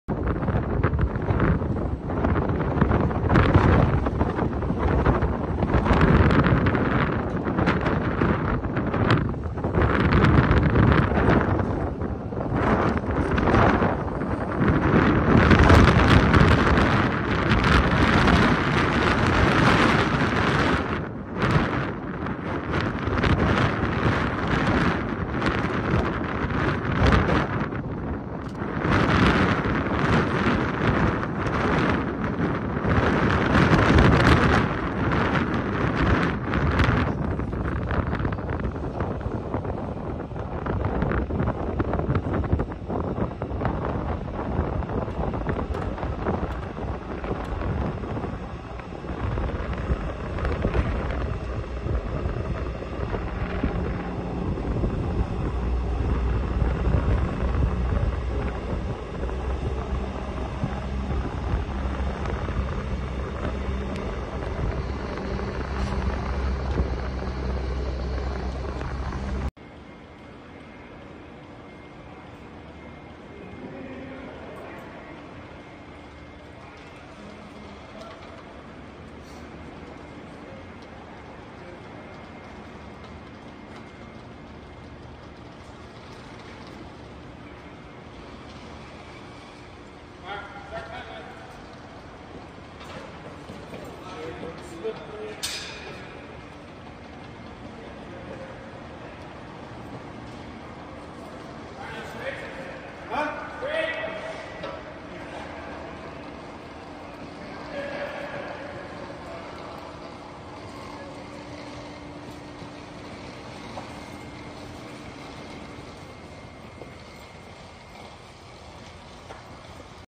Engine Extraction: The 191st Maintenance Squadron's F108 Jet Engine Removal
U.S. Airmen of the 191st Maintenance Squadron, Michigan Air National Guard, remove an F108 jet engine from a KC-135T Stratotanker aircraft, Feb. 6, 2024 at Selfridge Air National Guard Base, Michigan.

Selfridge Air National Guard Base Airmen Remove Jet Engine from KC-135T Aircraft
SELFRIDGE AIR NATIONAL GUARD BASE, MI, UNITED STATES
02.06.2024

Jet engines are replaced as part of periodic maintenance and to conduct repairs. (U.S. Air National Guard Video by Tom Demerly)

Film Credits: Video by Tom Demerly
127th Wing Public Affairs